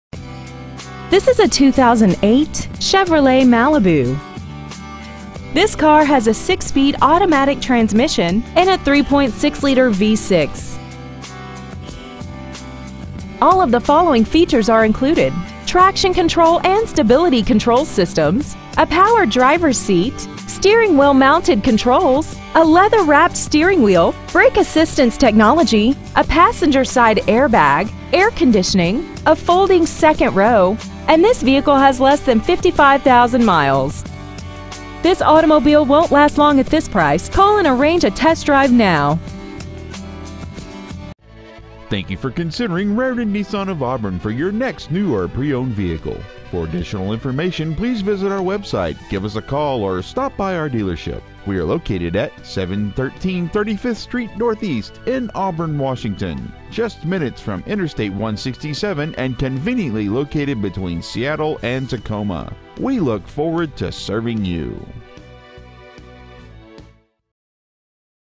2008 Chevrolet Malibu Auburn WA 98002
Year : 2008
 Make : Chevrolet
 Model : Malibu
 Engine : 3.6L V6
 Trans . : 6 Spd Automatic
 Exterior : Black Granite Metallic
 Miles : 54,694

 Stock : 8F278996